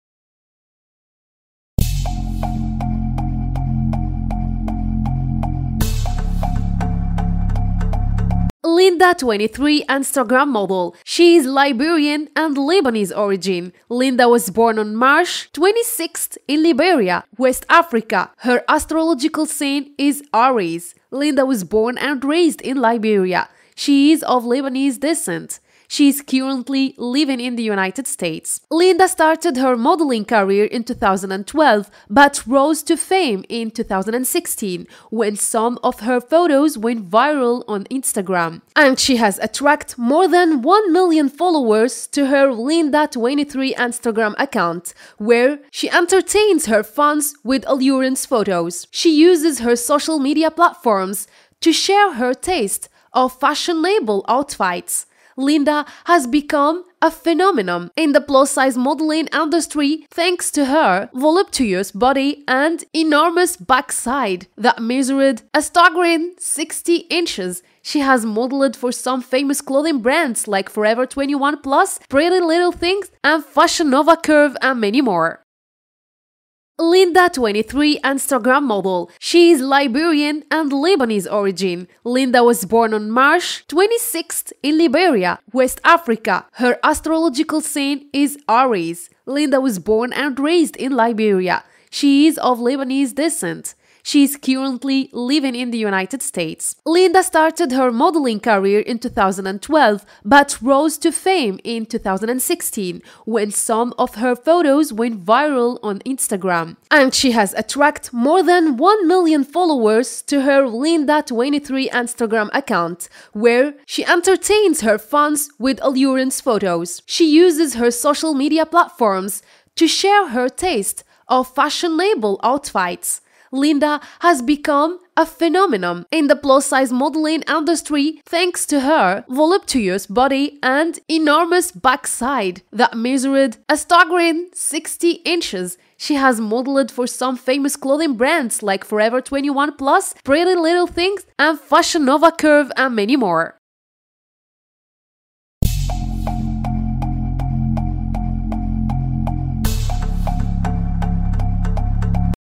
llindaa .. Curvy & Wiki biography - Body Positivity - Curvy Haul Fashion
llindaa23 Instagram Model Fashion nova & Biography and Lifestyle

 llindaa23 sports two swimsuit looks from the Fashion Nova Curve collection.

ـــــــــــــــــــــــــــــــــــــــــــــــــــــــــــــــــــــــــــــــــــــــــ
LEGAL INFORMATION ABOUT THE CONTENT:
ــــــــــــــــــــــــــــــــــــــــــــــــــــــــــــــــــــــــــــــــــــــــــ